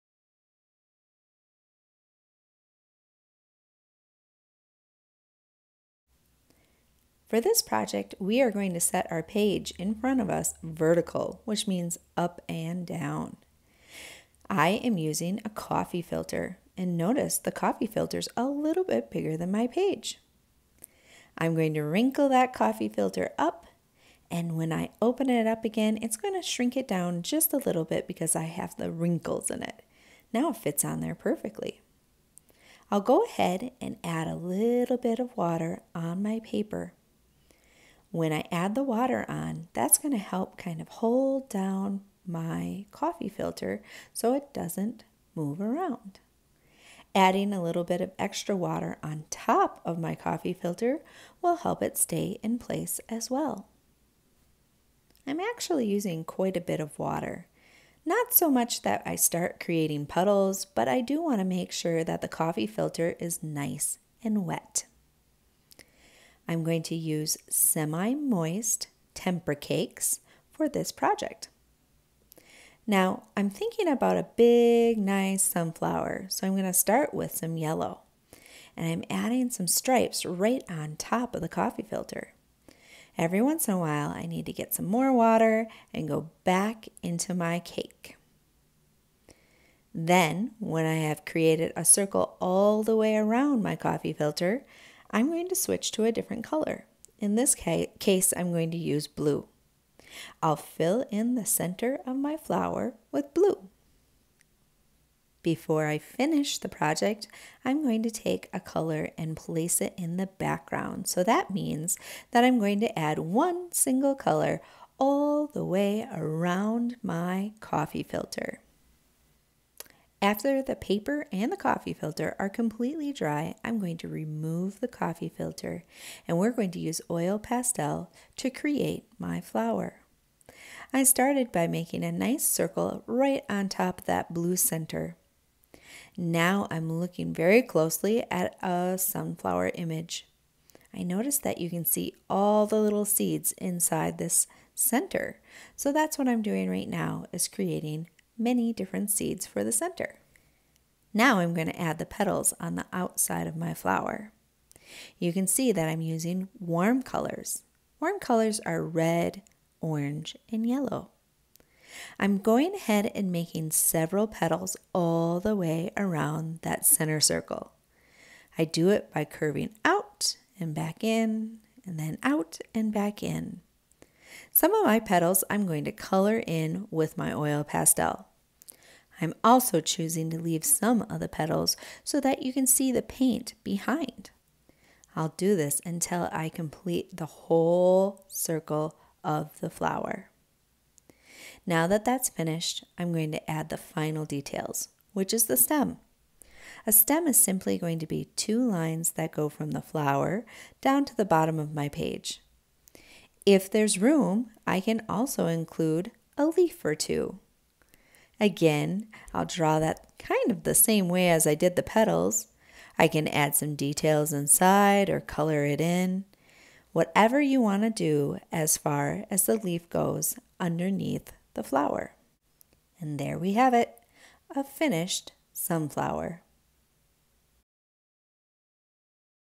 Sunflower
This lesson is intended for the beginner Artist. I have created this lesson for my Art to Remember fundraiser this year.